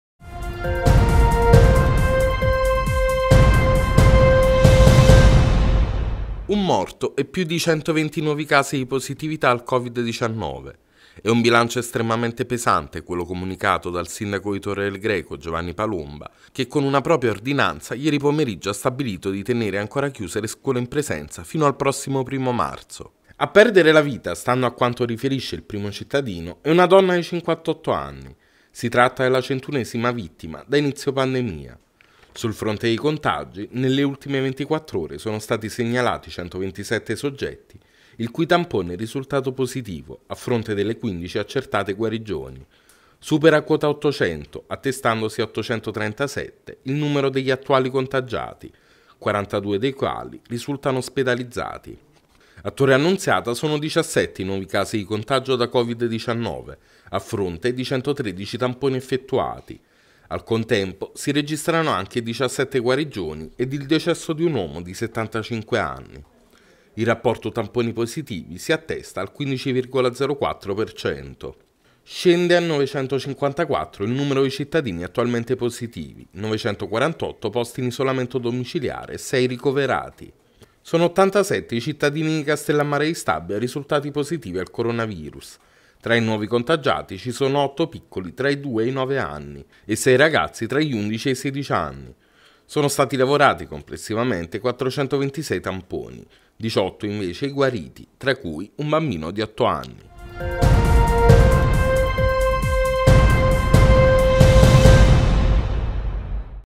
Covid nel Vesuviano: nuovo boom di contagi a Torre del Greco
Un morto e più di 120 nuovi casi di positività al Covid-19. È un bilancio estremamente pesante quello comunicato dal sindaco di Torre del Greco, Giovanni Palomba, che con una propria ordinanza ieri pomeriggio ha stabilito di tenere ancora chiuse le scuole in presenza fino al prossimo primo marzo.
A perdere la vita, stando a quanto riferisce il primo cittadino, è una donna di 58 anni: si tratta della centunesima vittima da inizio pandemia. Sul fronte dei contagi, nelle ultime 24 ore sono stati segnalati 127 soggetti il cui tampone è risultato positivo, a fronte delle quindici accertate guarigioni. Supera quota 800, attestandosi a 837, il numero degli attuali contagiati, 42 dei quali risultano ospedalizzati.
A Torre Annunziata sono 17 i nuovi casi di contagio da Covid-19 a fronte di 113 tamponi effettuati. Al contempo si registrano anche 17 guarigioni ed il decesso di un uomo di 75 anni. Il rapporto tamponi/positivi si attesta al 15,04 per cento.
Scende 954 il numero di cittadini attualmente positivi: 948 posti in isolamento domiciliare e 6 ricoverati.
Sono 87 i cittadini di Castellammare di Stabia risultati positivi al coronavirus. Tra i nuovi contagiati ci sono otto piccoli tra i 2 ed i 9 anni e sei ragazzi tra gli 11 ed i 16 anni. Sono stati lavorati complessivamente 426 tamponi. 18, invece, i guariti, tra cui un bambino di 8 anni.